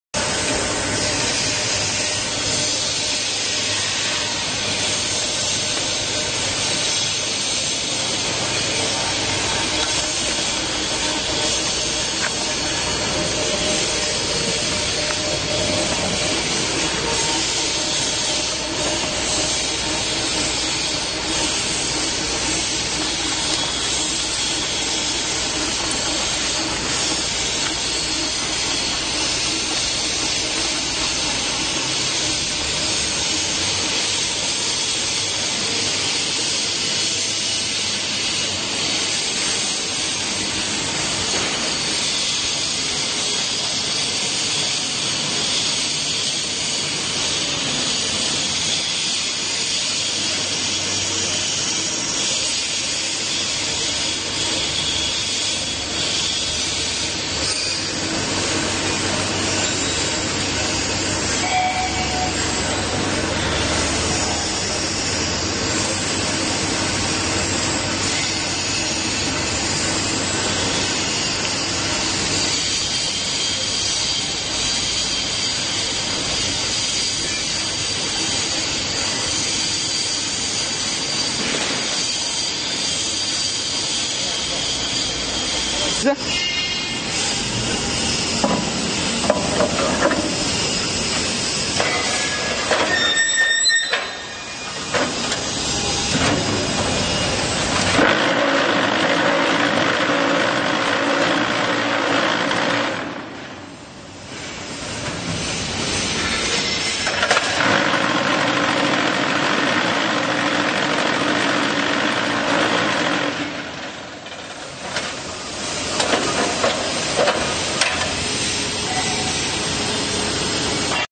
QT4 25 Semi automatic concrete block making machine
QT4-25 is one semi automatic concrete block machine, can be all types of hollow blocks, solid blocks, pavers and Curbstones by changing the moulds, with the easy operation, stable working, and lower investment, attract many customer to buy it. if you have interest to this machine contact us Qingdao Darifeng Machinery Co.,Ltd, Email: , Whatsapp: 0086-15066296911,